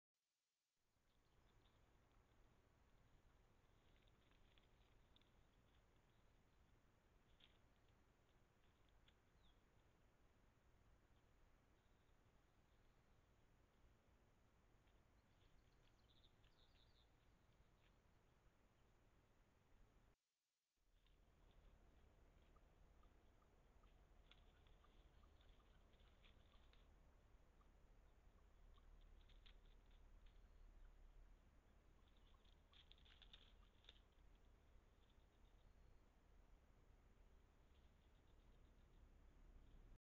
PAREJA DE PERDICES ROJAS ( Alectoris rufa )
Hola compañeros, por fin puedo subir algo, ahí va esa pareja de pájaros perdices, espero os guste.
Camara Hapimp.  4k